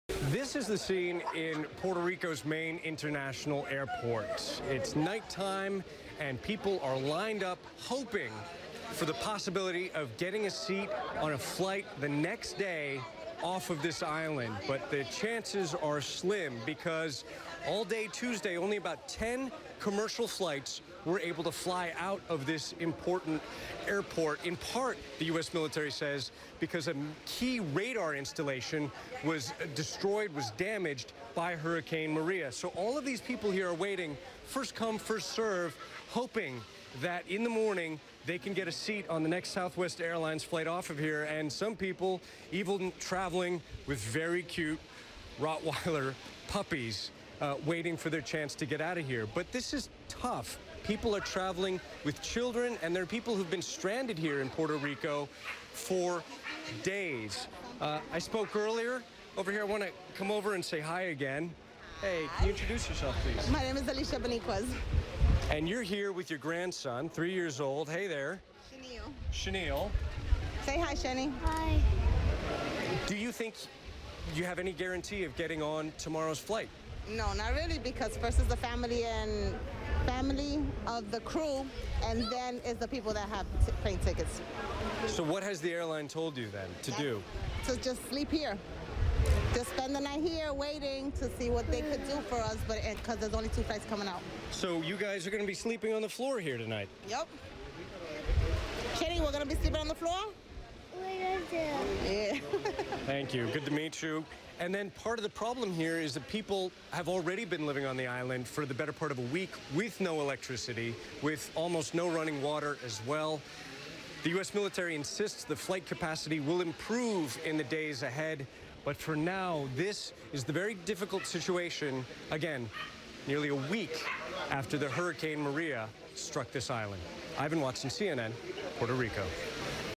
Hundreds stranded at Puerto Rican airport
Hundreds of passengers are stranded at the San Juan airport in Puerto Rico. CNN's Ivan Watson reports.